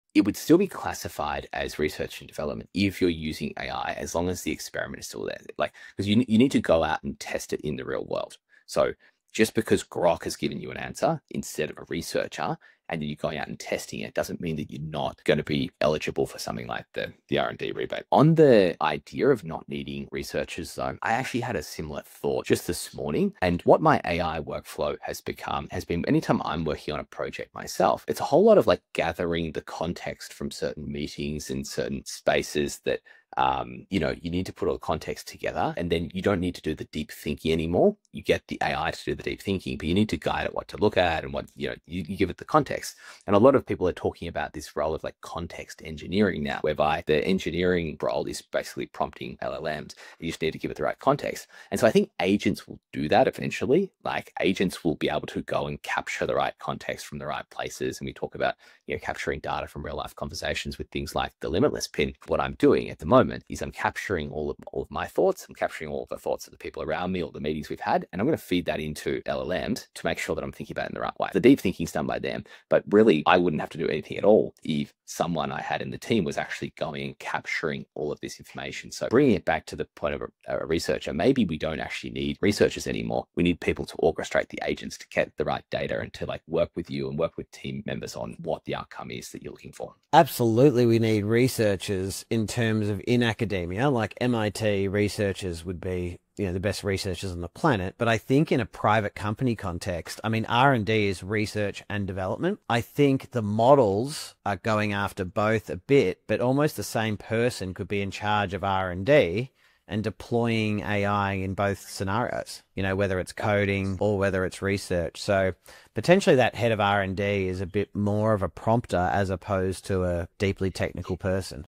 Research and Development using AI
𝗔𝗜 𝗶𝘀 𝗰𝗵𝗮𝗻𝗴𝗶𝗻𝗴 𝘁𝗵𝗲 𝗴𝗮𝗺𝗲 𝗳𝗼𝗿 𝗿𝗲𝘀𝗲𝗮𝗿𝗰𝗵 𝗮𝗻𝗱 𝗱𝗲𝘃𝗲𝗹𝗼𝗽𝗺𝗲𝗻𝘁 𝗶𝗻 𝗰𝗼𝗺𝗽𝗮𝗻𝗶𝗲𝘀.

Deep thinking and analysis AI is handling that now.

What companies need right now are people who can guide AI; giving the right context orchestrating agents and working with teams to get results.

The future head of R and D might be less about deep technical skills and more about mastering the art of prompting AI.